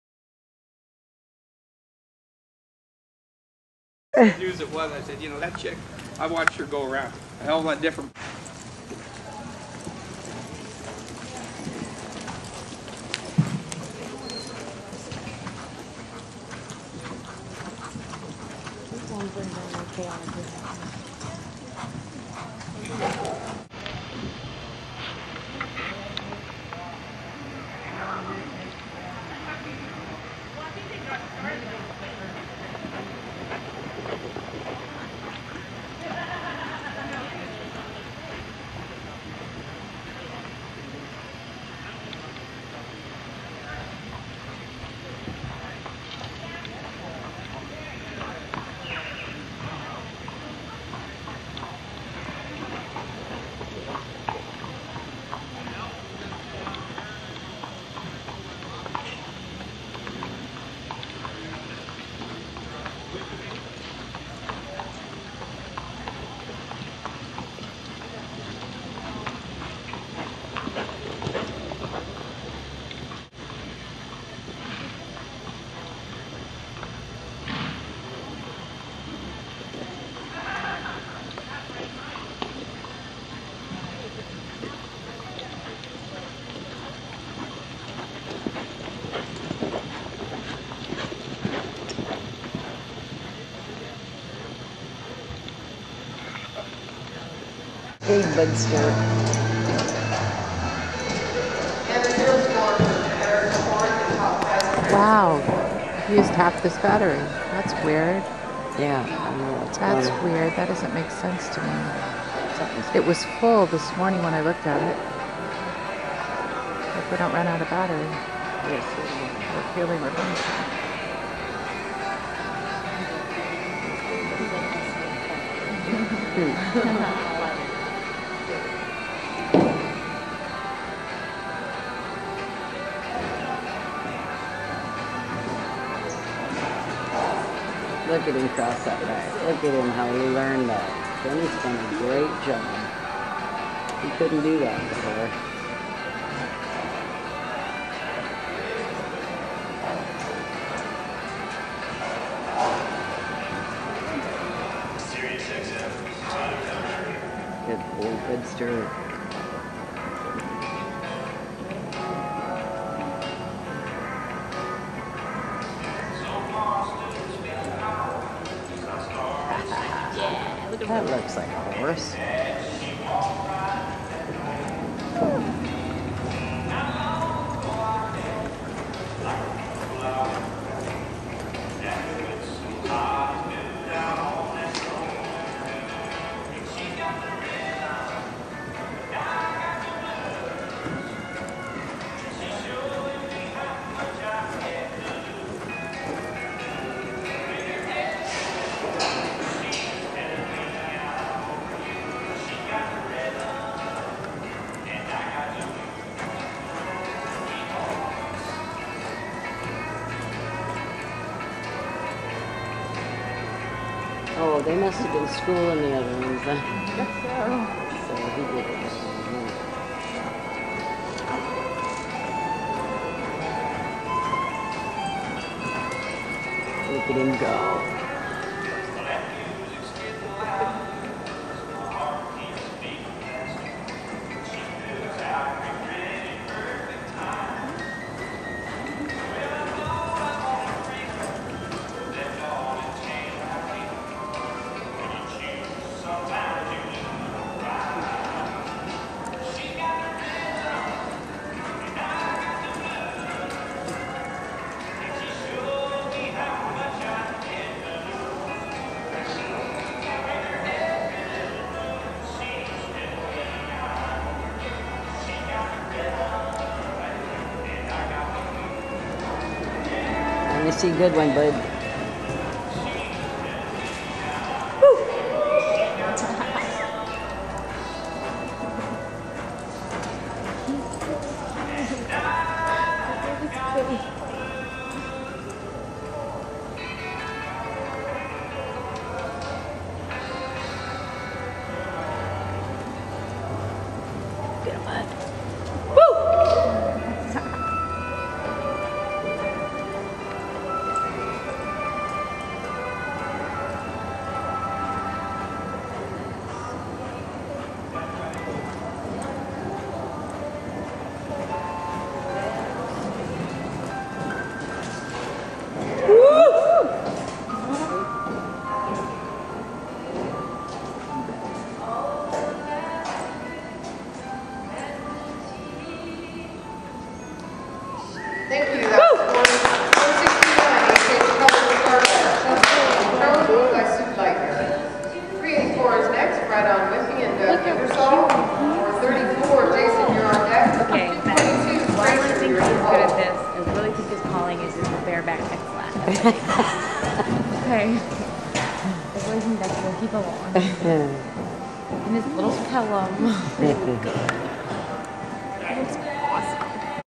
Buddy at MEC
HH Colored Starlight aka Buddy  Senior Reining MEC Feb 2013 ridden and trained by Benny Guitron, owned by Sue Lightner